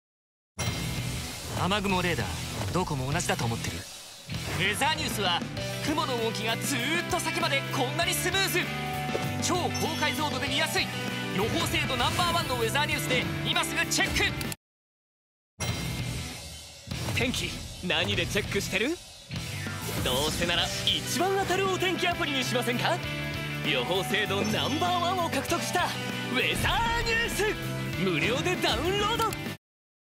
Voice Actors Hiroshi Kamiya and Yuki Kaji Featured!
Latest News☛

A new television commercial for the "Weather News" app featuring voice actors Hiroshi Kamiya and Yuki Kaji has been released. The campaign includes five different commercial patterns: the "Rainy Day" version with 17 variations tailored for specific areas, the "Sudden Rain" version also with 17 area-specific variations, and the "Weekend Weather" version which is a single nationwide spot. In total, there are 37 different commercials set to be broadcast optimally according to weather conditions and geographic regions.